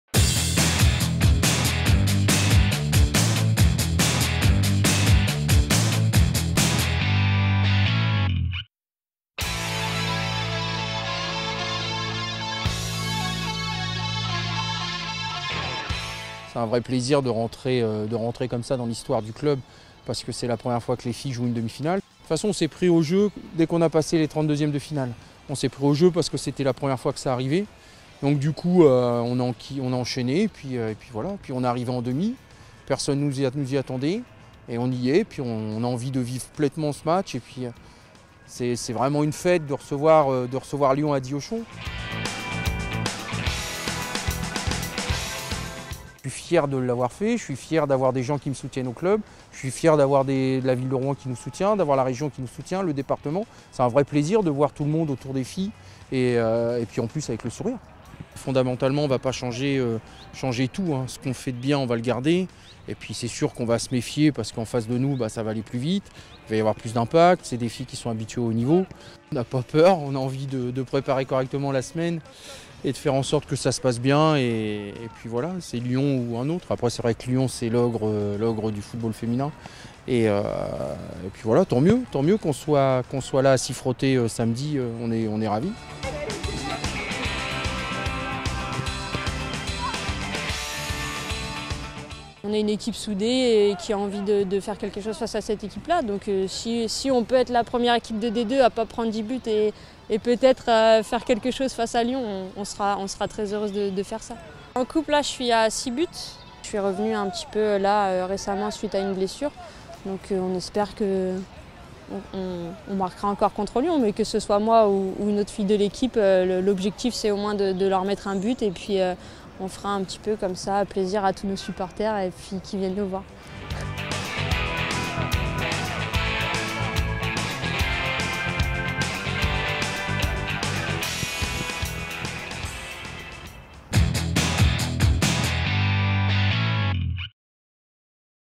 Le FC Rouen section féminine en demie-finale de la Coupe de France
Une grande première dans l'Histoire du FC Rouen !
Pour la première fois, la section féminine de D2 joue une demie-finale de Coupe de France.
L'équipe affrontera l'Olympique Lyonnais Samedi 14 mars au stade Diochon de Rouen.